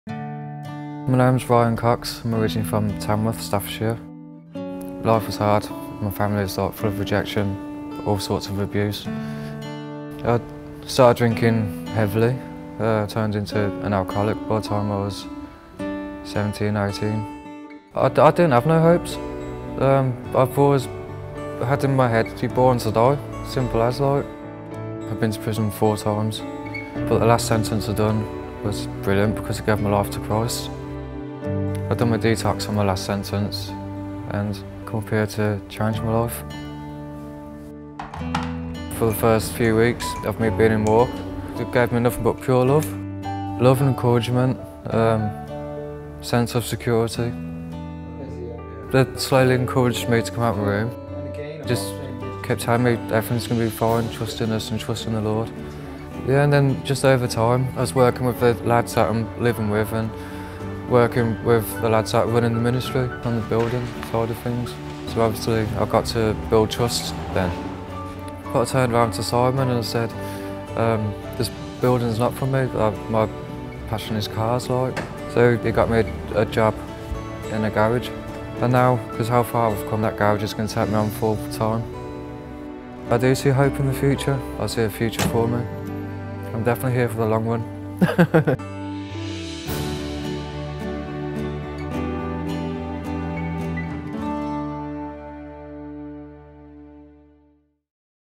Ryan's Story
Already an alcoholic by the time he was a teenager and having spent four spells in prison, Ryan found hope through a project supported by Church Urban Fund, now he's working in his dream job.